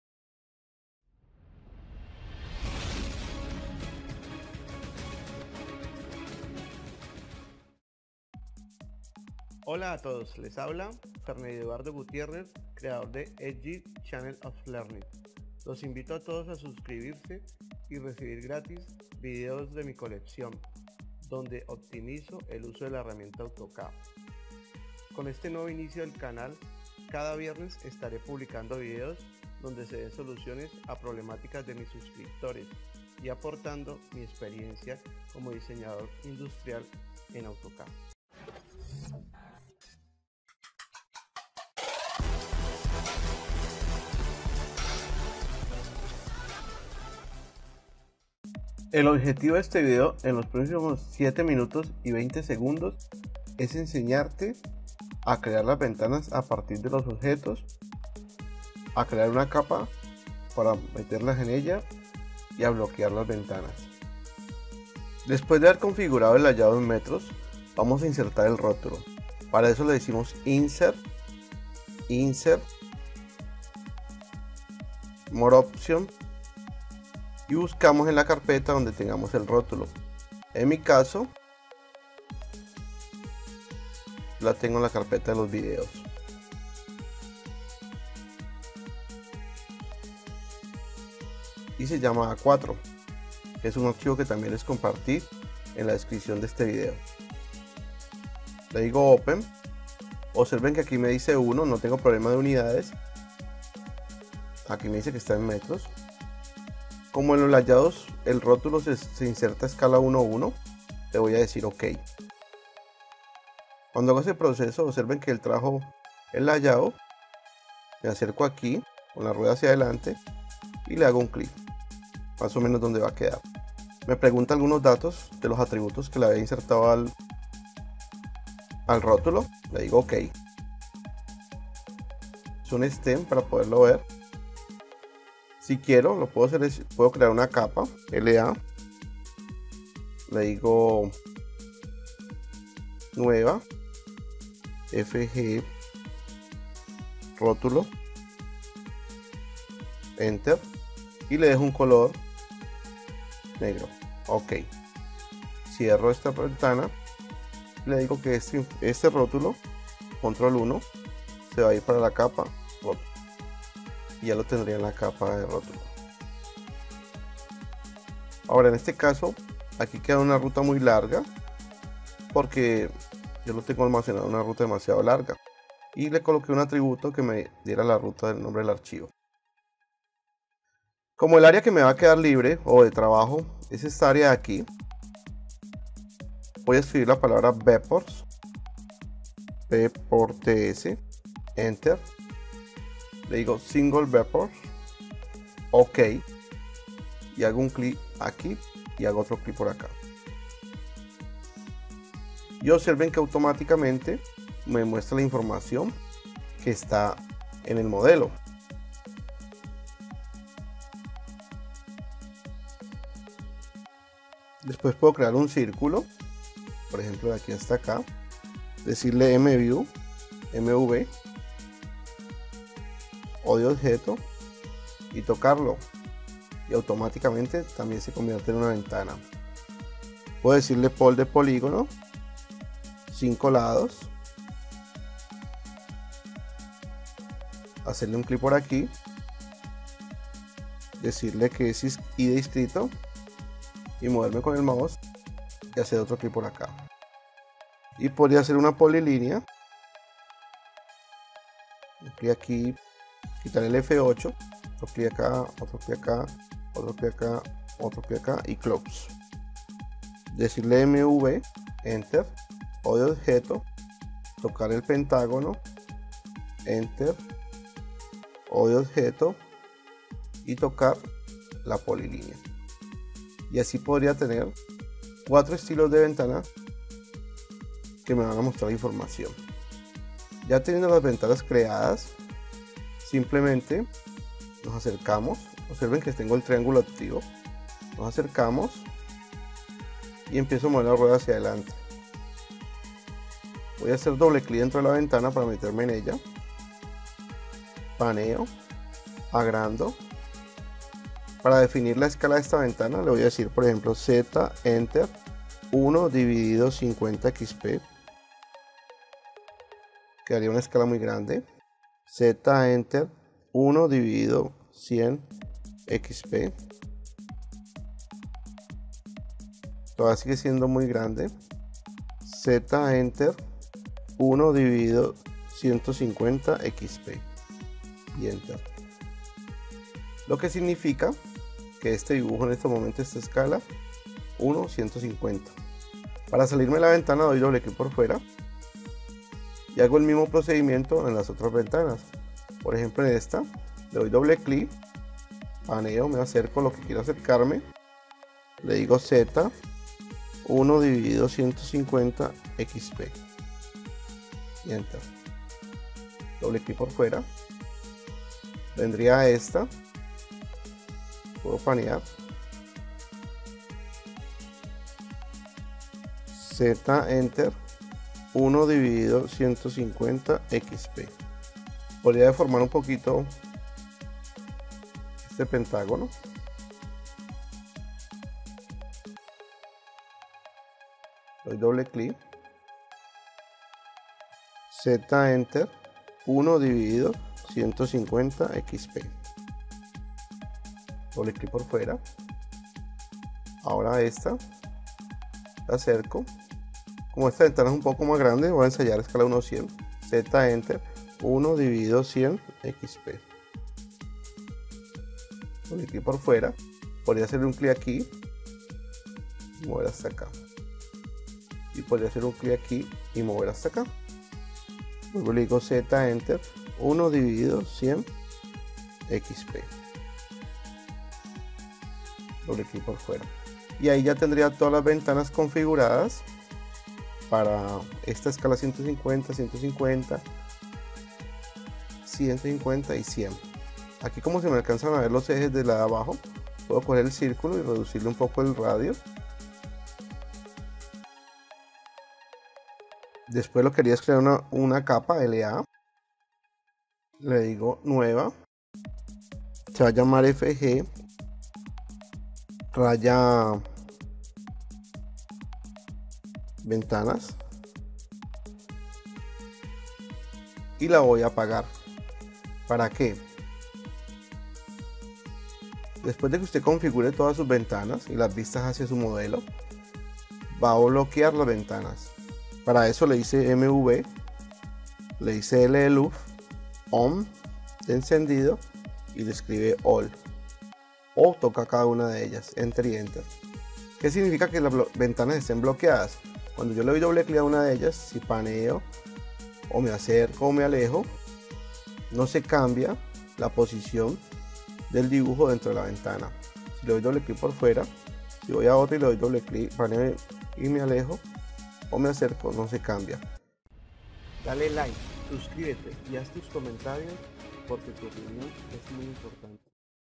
Crear Ventanas en el Layout de AutoCAD 2018
DESCRIPCIÓN
El objetivo de este vídeo es enseñarte en los próximos 7 minutos y 20 segundos. Es enseñarte crear ventanas rectangulares, circulares, pentagonales y poligonales así como configura sus escala en AutoCAD 2018

Agradecimientos a por el intro del video.

Contacto Para Asesorías, Seminarios Y Cursos Personalizados (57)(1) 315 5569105